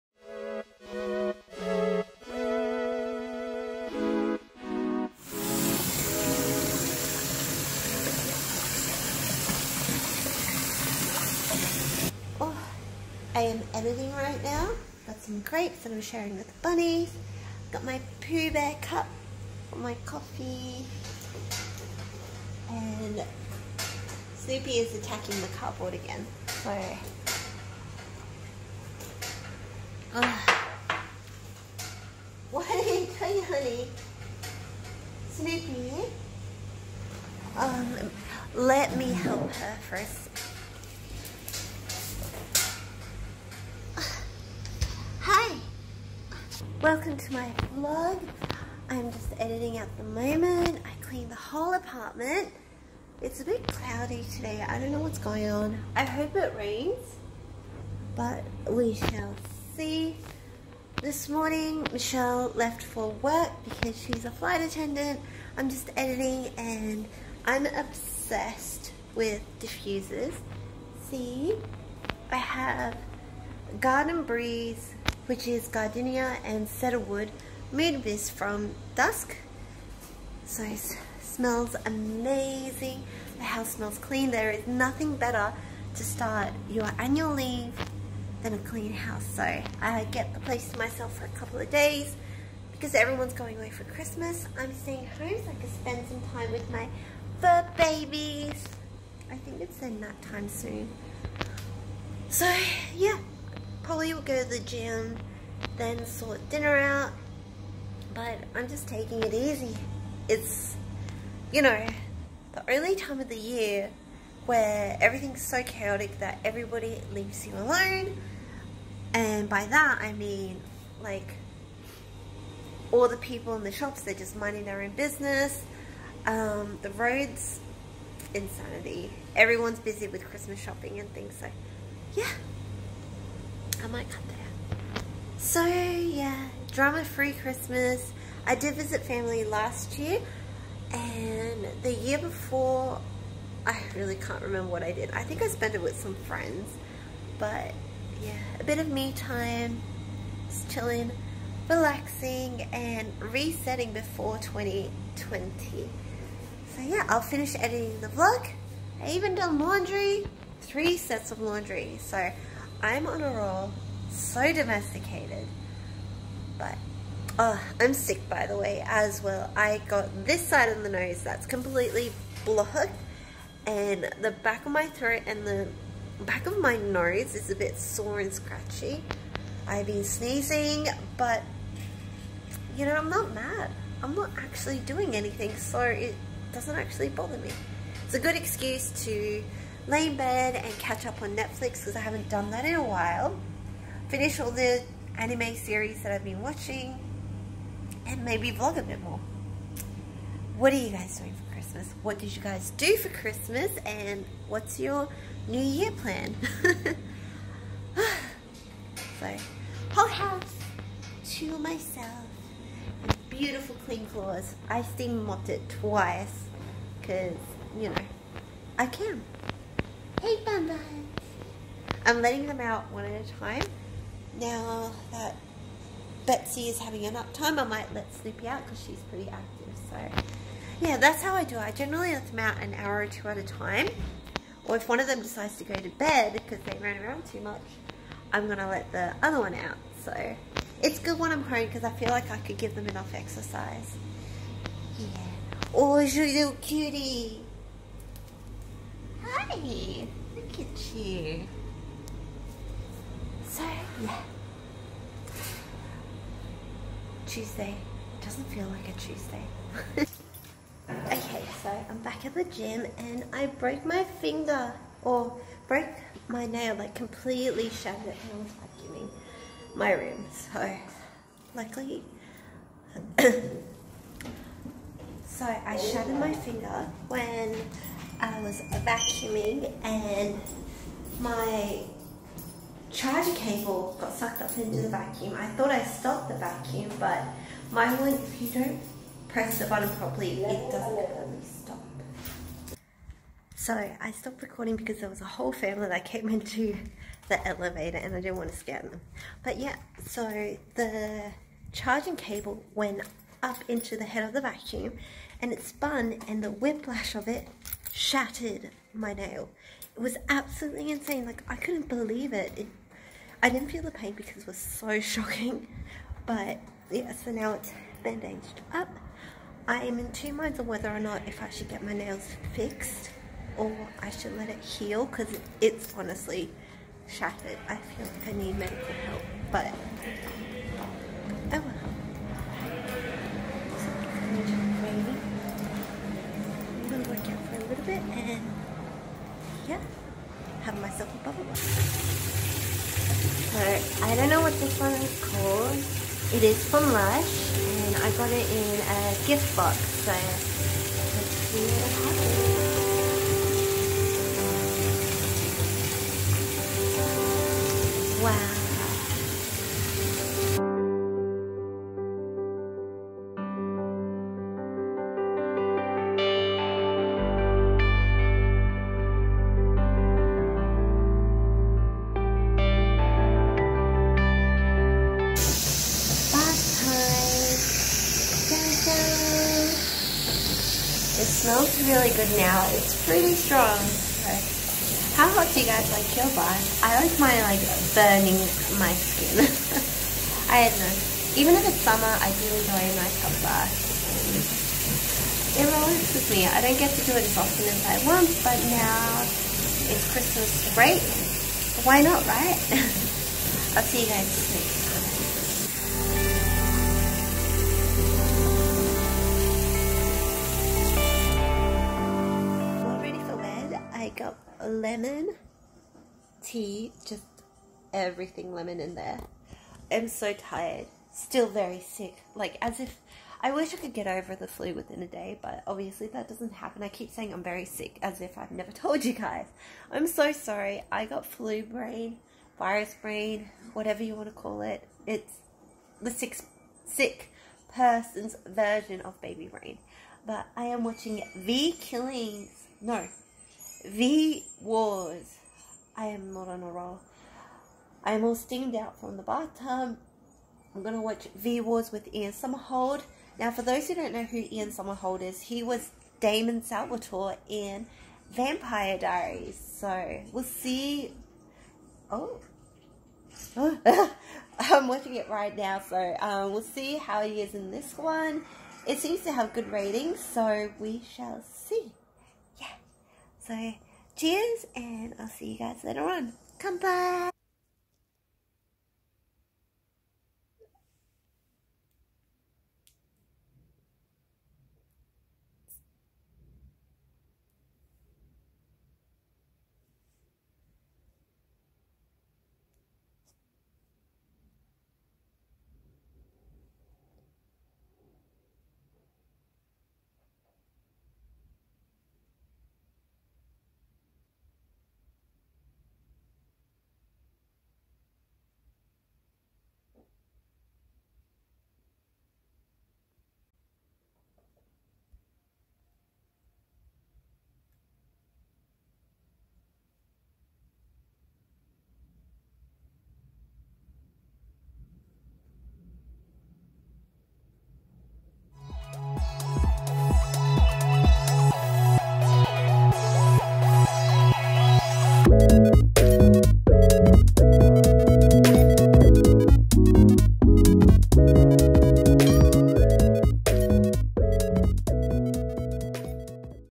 I'm sick on Xmas! - XMAS VLOG
XMAS VLOG!

I’m sick on Christmas… so i stayed home. I had my bunnies keeping me entertained though. Snoopy is loving the mirror in my room these days. Betsy just chills quietly.

Nothing is better than a nice warm bath to help recover!

Hope you all had a wonderful holiday season!

My TIKTOK 👉 itschoolie